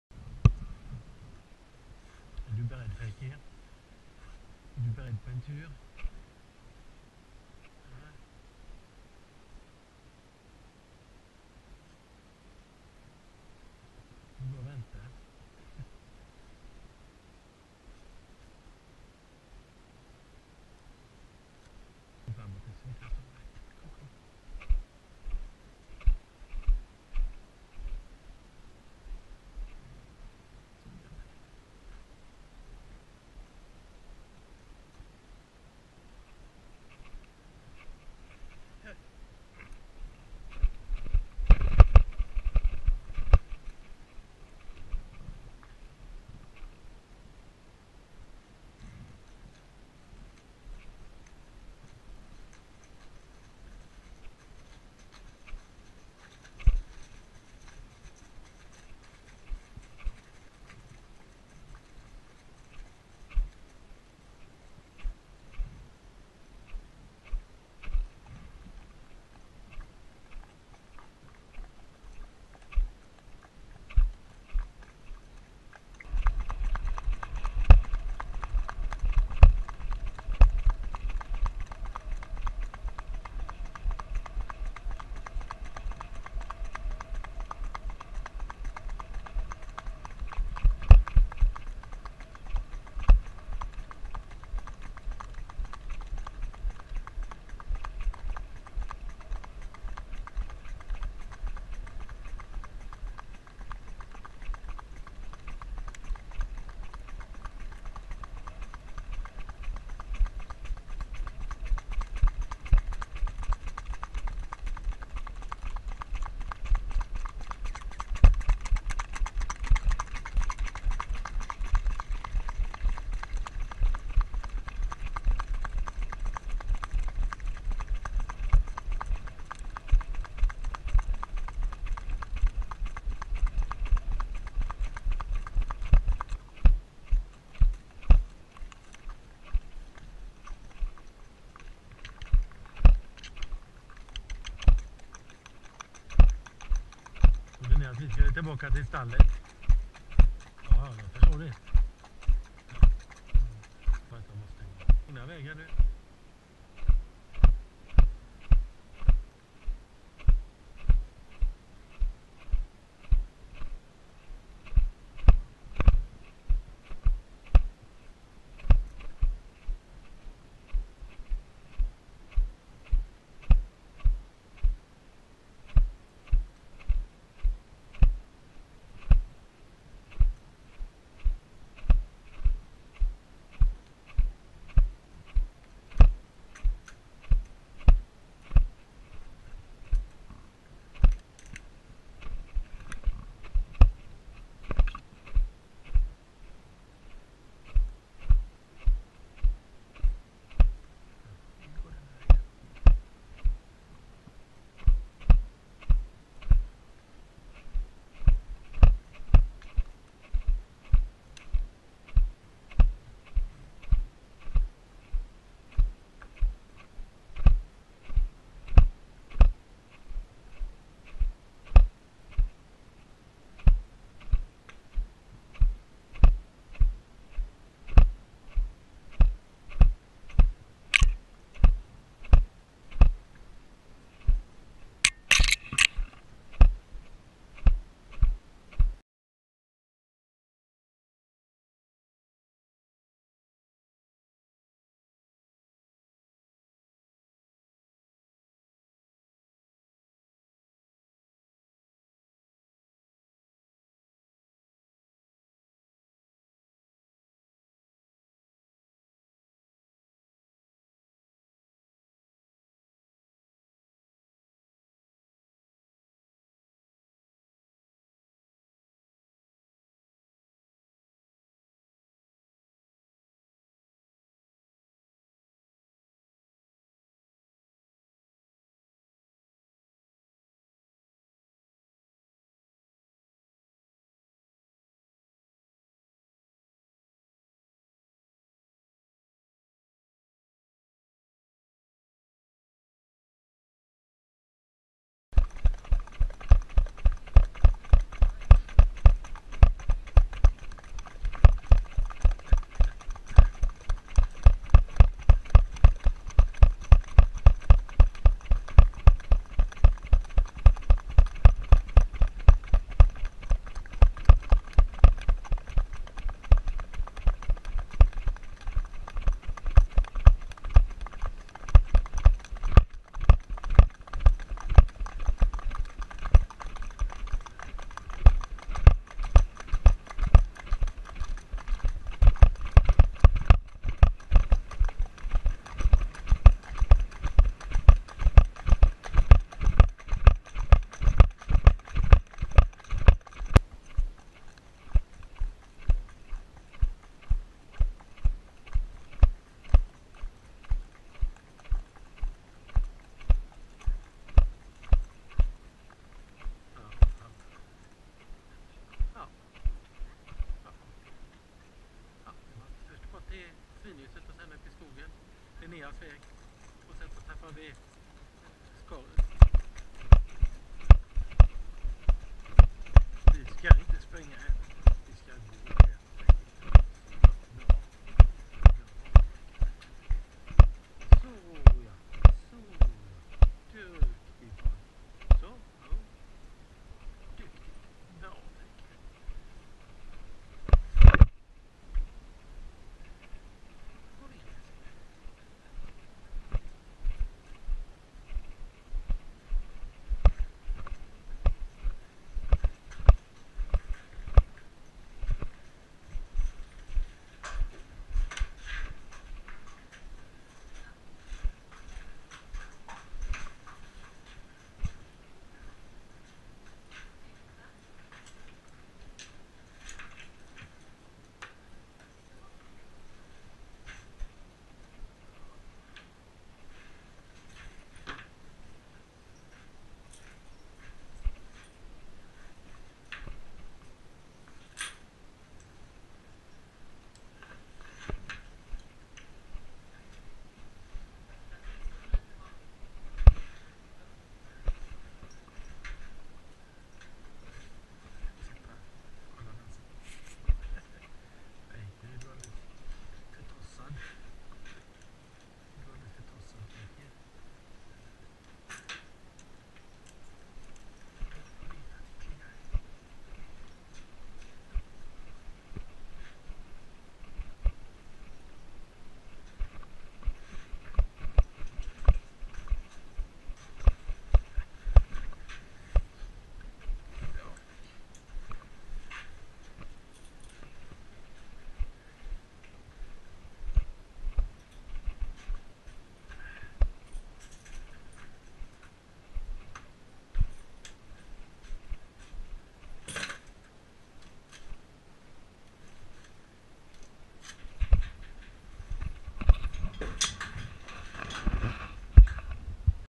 Tjaldur Feykir Jim Linnea
Första testet av Gopro Hero kameran. tyvärr så lossade den lite från
hjälmen och ett klickande ljud uppstod. Men till nästa gång är detta
justerat